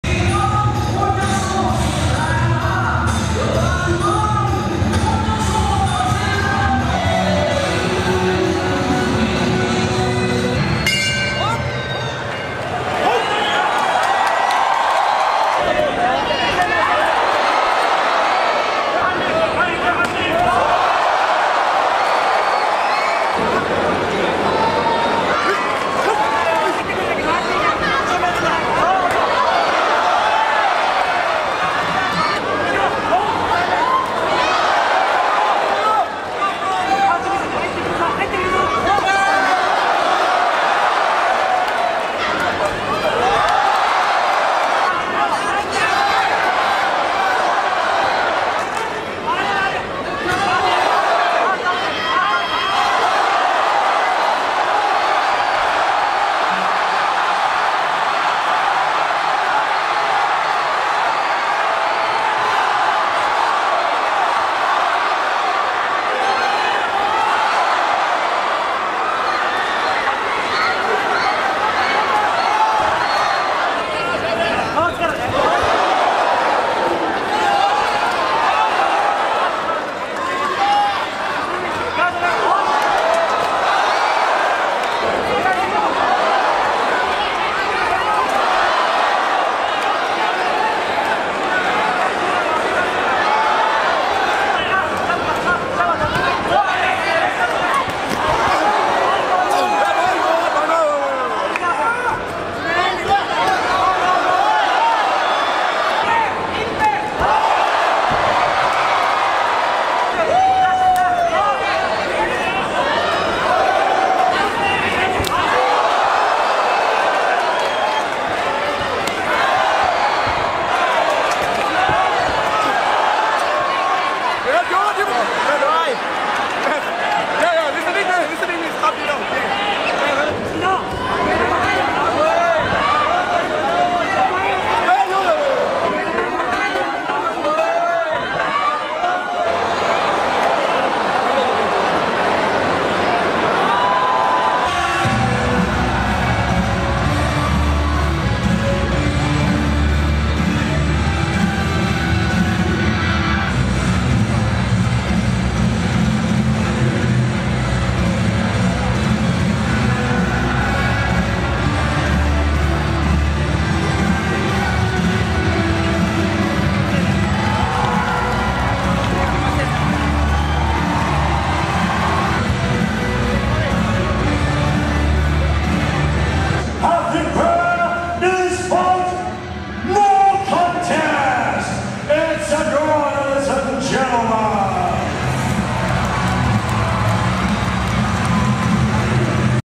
Buakaw vs. Yasuhiro Kido | RWS Legend Of Rajadamnern
Buakaw and Yasuhiro Kido step into the ring at the famous Rajadamnern Stadium in Bangkok for RWS.